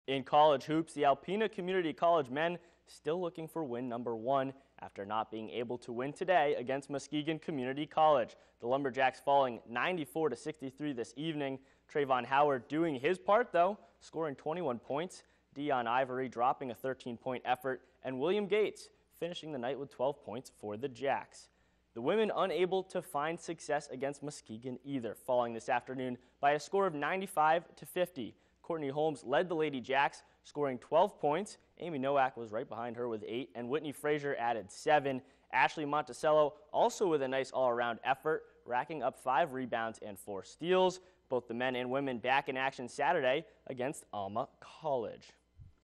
ACC men, women fall to Muskegon CC
Alpena unable to beat Muskegon in men's and women's hoops.